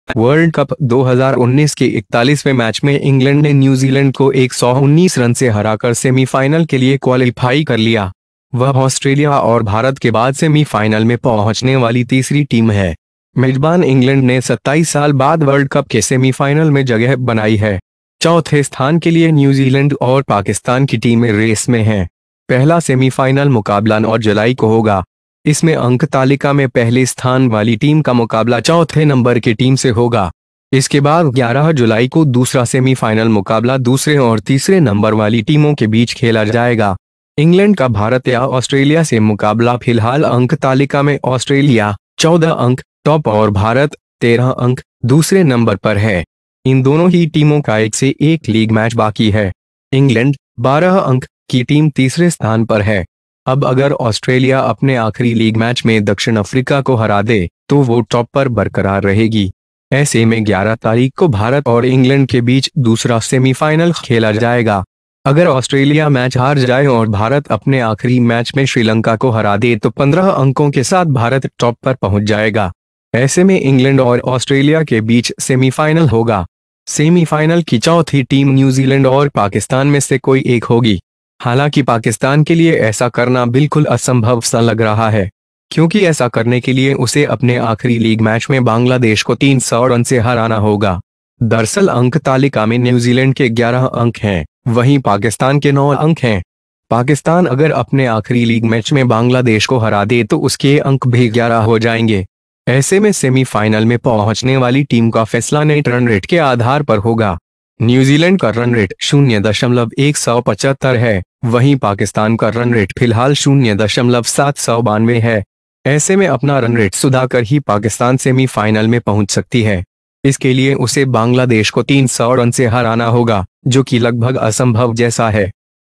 इंग्लैंड का सेमीफाइनल मुकाबला भारत या ऑस्ट्रेलिया से
In the 41st match of the World Cup 2019 England defeated New Zealand by 119 runs to qualify for the semi-finals. He is the third team to reach the semifinals after Australia and India. Host England has made it to the semi-finals of the World Cup 27 years later. New Zealand and Pakistan teams are in the race for the fourth position. The first semifinal match will be on July 9. In this, the team with the first place in the points table will face the fourth team. After this, the second semifinal match will be played between the teams of the second and third teams on July 11. The current number of matches against India or Australia is Australia (14 points) and India (13 points). Both of these teams have 1-1 league match left. England (12 points) team is in third position. Now if Australia beat South Africa in their last league match, then they will remain on top. In this case, the second semi-final will be played between India and England on 11th. If Australia lose the match and India beat Sri Lanka in their last match, then India will go top with 15 points. In this case, there will be semifinal between England and Australia. Fourth team of semi-finals will be one of New Zealand and Pakistan. Although it seems impossible to do this for Pakistan. Because to do this, he will have to beat Bangladesh by 300 runs in his last league match. In fact, New Zealand has 11 points in the points table, while Pakistan has 9 points. If Pakistan beat Bangladesh in its last league match then its points will also be 11. In such a case, the team reaching the semi-finals will be decided on the basis of net runrets. New Zealand's run-rate is 0 decimal 175, while Pakistan's run rate is currently 0 decimal at 792. In such a situation, only after recovering his run-rate, Pakistan can reach the semi-finals. For this, he will have to beat Bangladesh by 300 runs, which is almost impossible.